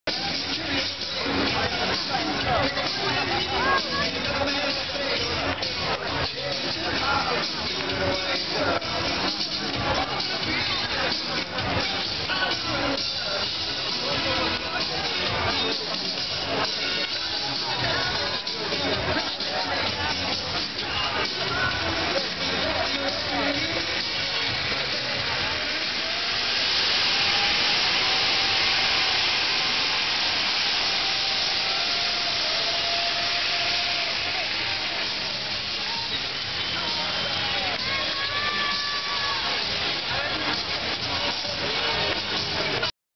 Taggia Falò in Piazza Cavour Da Hotel Lucciola
La festa tradizionale di San Benedetto Revelli a Taggia il 13 febbraio 2010. In particolare i furgari ed il falò di Piazza Cavour nel Centro Storico. Siamo in provincia di Imperia a 10 km da Sanremo. Da Albergo Lucciola di Santo Stefano al Mare tel. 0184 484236